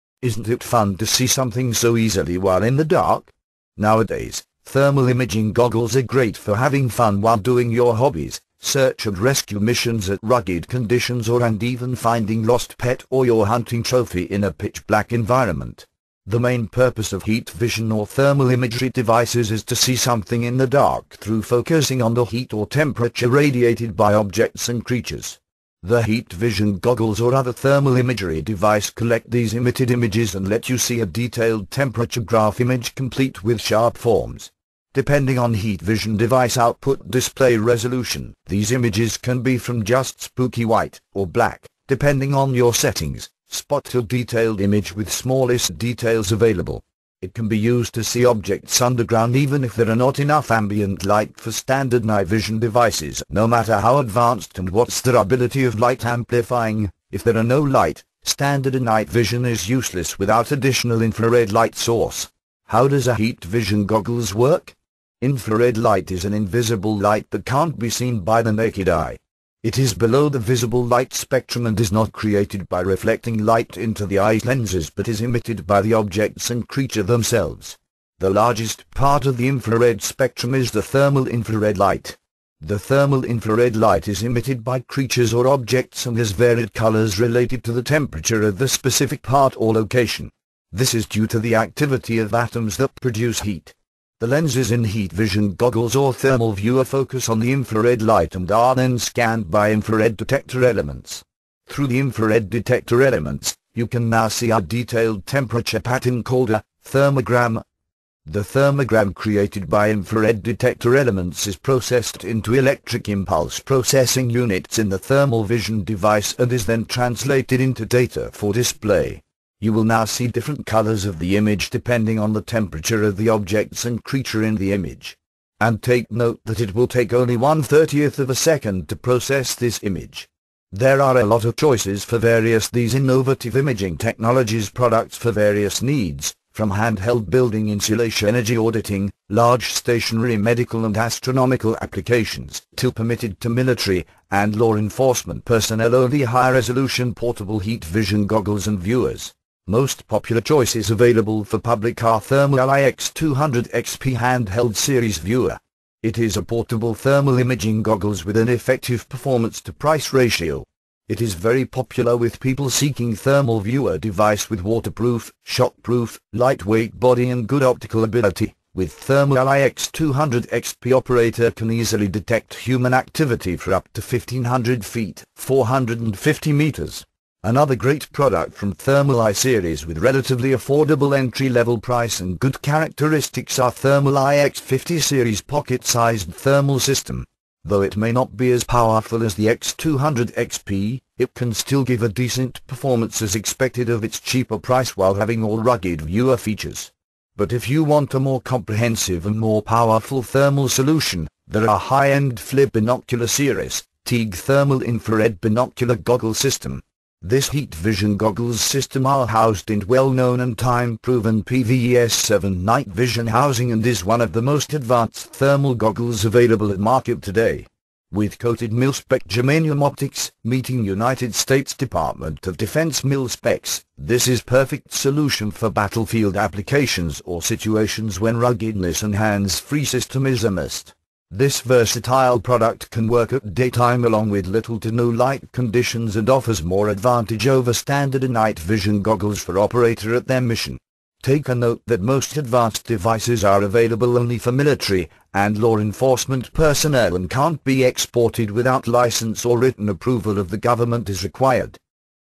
Heat Vision Goggles -- New Generation of Thermal Imagery
Isn't it fun to see something so easily while in the dark? Nowadays, thermal imaging goggles are great for having fun while doing your hobbies, search and rescue missions at rugged conditions or and even finding lost pet or your hunting trophy in a pitch black environment. The main purpose of heat vision or thermal imagery devices is to see something in the dark through focusing on the heat or temperature radiated by objects and creatures. Check our site NightVision.Lv for more information!